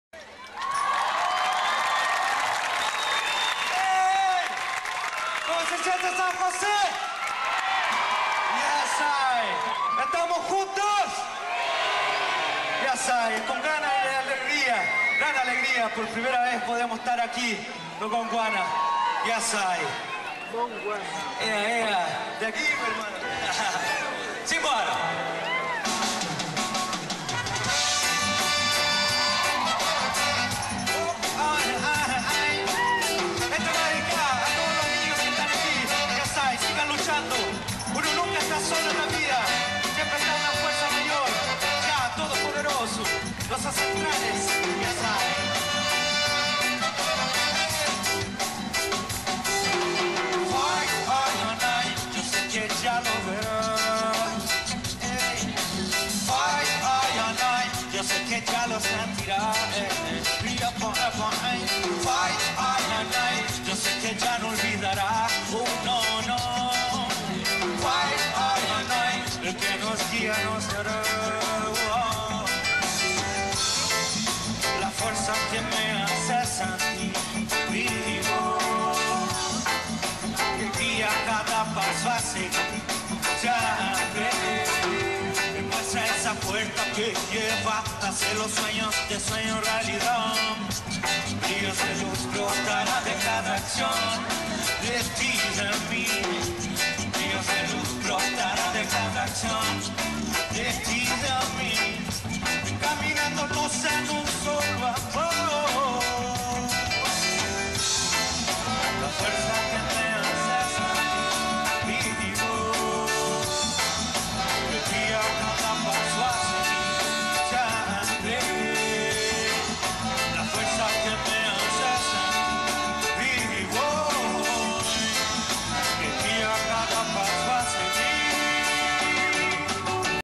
San José de la Mariquina - GONDWANA [Fight]
02 de diciembre del 2005: Presentación de GONDWANA en San José de la Mariquina - Región de los Ríos